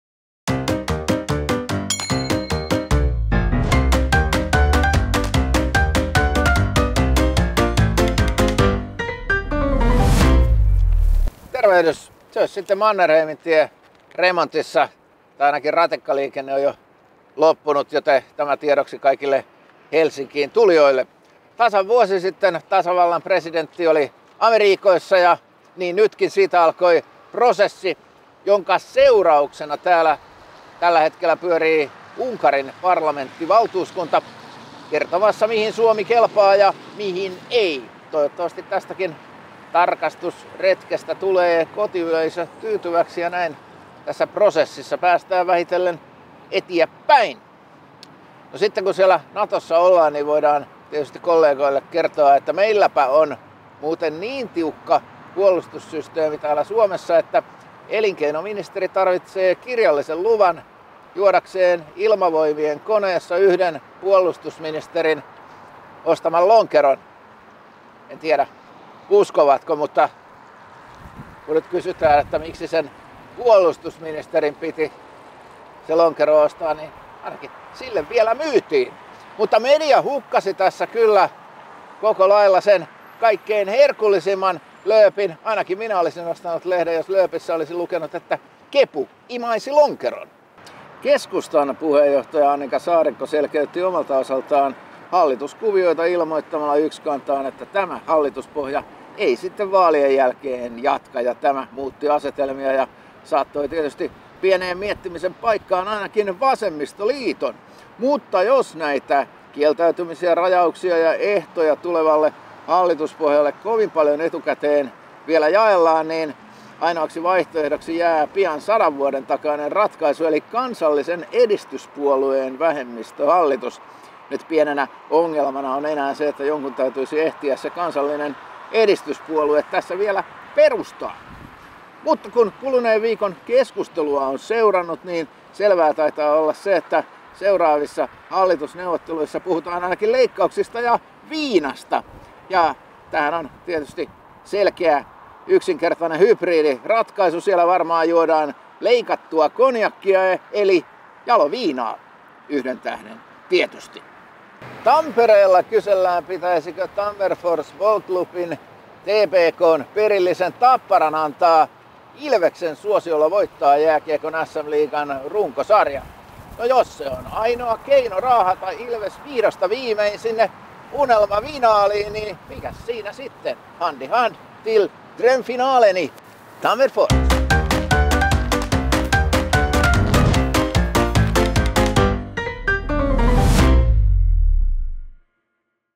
KorkkiTV: Ainoa hallitusvaihtoehto saattaa löytyä sadan vuoden takaa
Jos kieltäytymisiä, ehtoja tai rajoituksia tulee vielä lisää, hallituspohjaa on vaikea löytää.